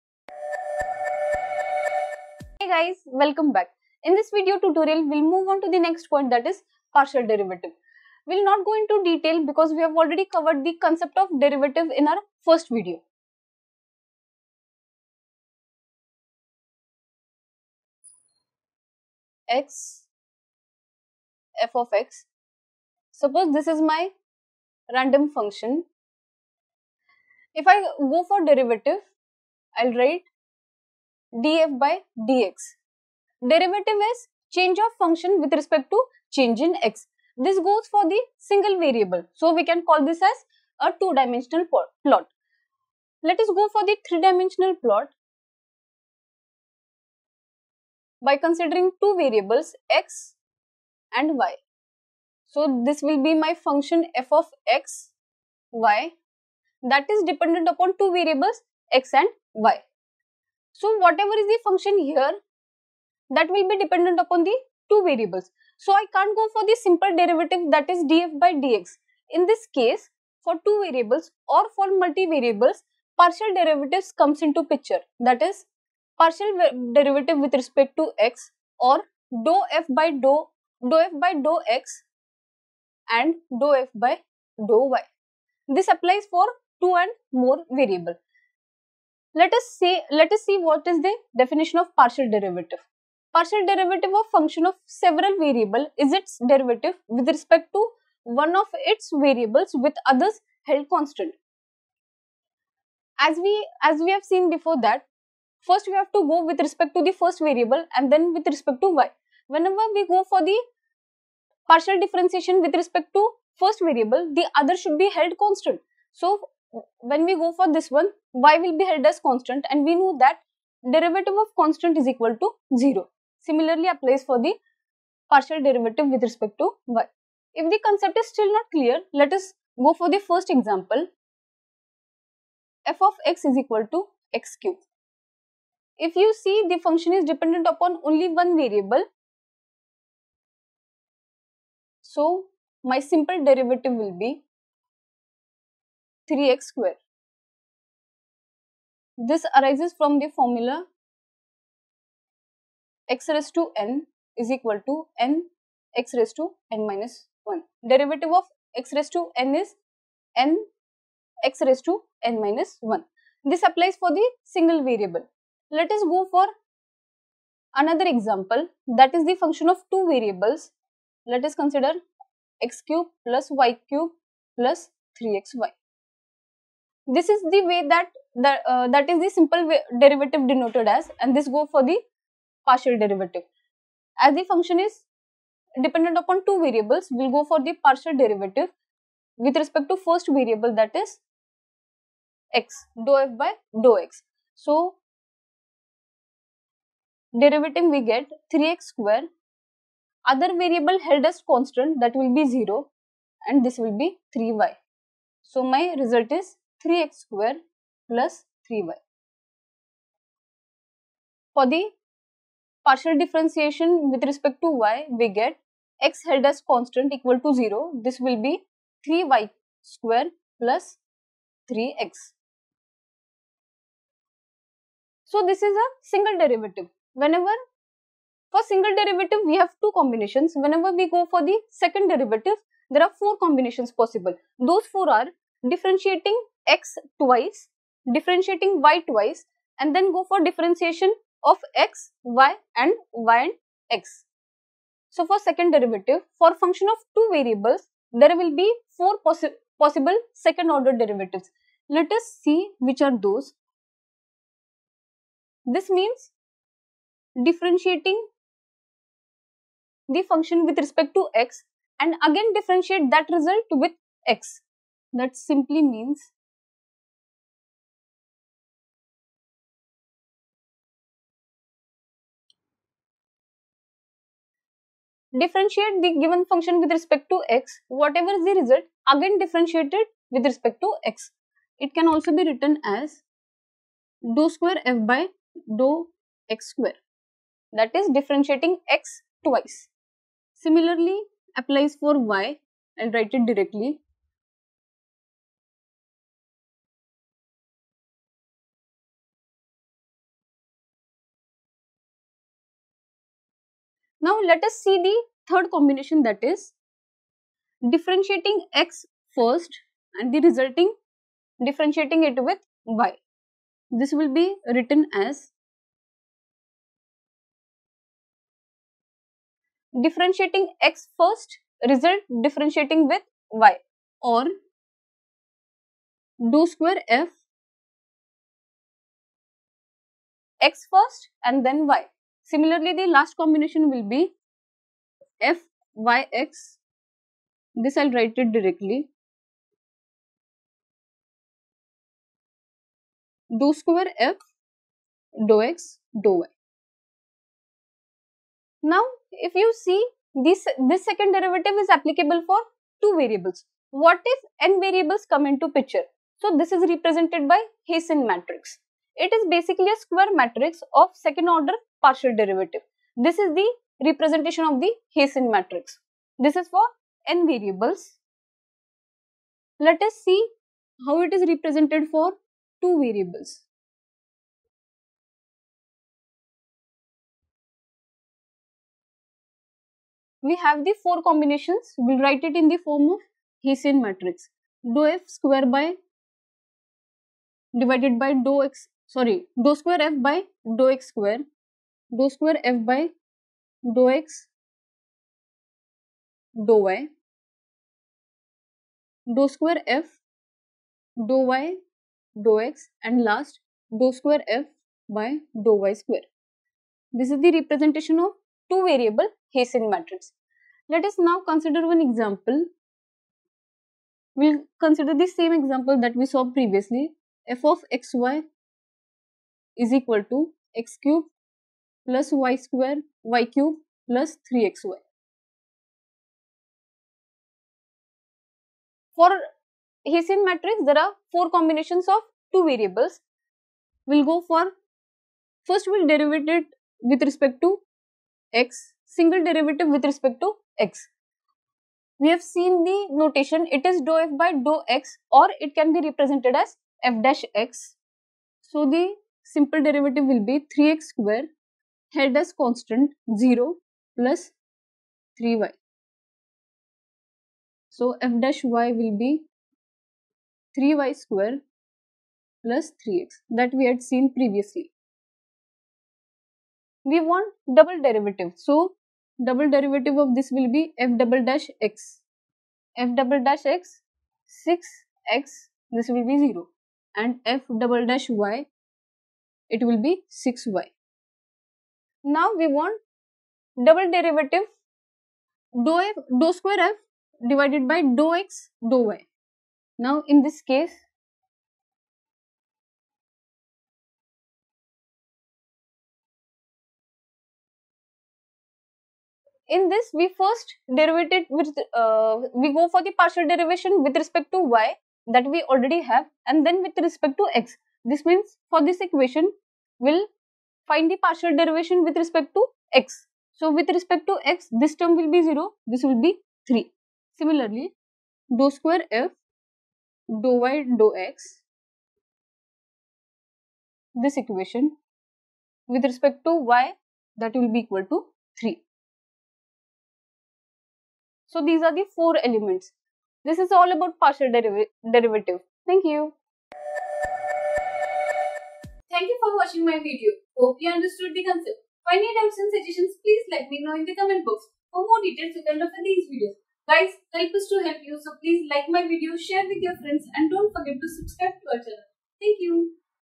6]Partial Derivative with Examples - Calculus - Engineering Mathematics
In this video tutorial, next concept “Partial Derivative” will be explained.
The concept of simple derivative is explained in 1st video.

Derivative, simply, means change in function with respect to variable (say x).
In two dimensional plot, function f(x) is dependent on single variable x. So, derivative is calculated or denoted as df/dx.
If two variables are considered, function f(x,y) is dependent on two variables x and y. If the function is dependent on two variables or multiple variables, partial derivative needs to be calculated. Partial derivative is represented or denoted as as ∂f/∂x i.e. partial derivative wrt x and ∂f/∂y i.e. partial derivative wrt y.

Definition of Partial Derivative:
Partial Derivative of function of several variable is its derivative wrt one of its variables, with others held constant.

Example on simple derivative is explained. Example is f(x) = x3
Formula used for calculation of simple derivative is also mentioned.

One more example based on partial derivative is explained. Example is f(x,y) = x3 + y3 + 3xy
Above examples are single derivative. For single derivative, two combinations are possible.

Second derivative:
For second derivative, four combinations are possible.
The four possible combinations are:
Differentiating x twice, differentiating y twice, differentiation of x and y, differentiation of y and x.

For function of two variables, there will be four possible second order derivatives.
(fx)x simply means differentiating function with respect to x and again differentiating that result with x. Same applies for (fy)y.

 Third combination (fx)y: differentiating x first and result differentiating with respect to y.
Last combination is  (fy)x
Second derivative is applicable for 2 variables.
For n variables, hessian matrix is used.

Hessian matrix:
Hessian matrix is basically a square matrix of second order partial derivative. For n variables, hessian matrix is represented as explained in the tutorial. Also, representation for 2 variables is explained.

Example based on Hessian matrix is explained. Example is x^3 + y^3 + 3xy.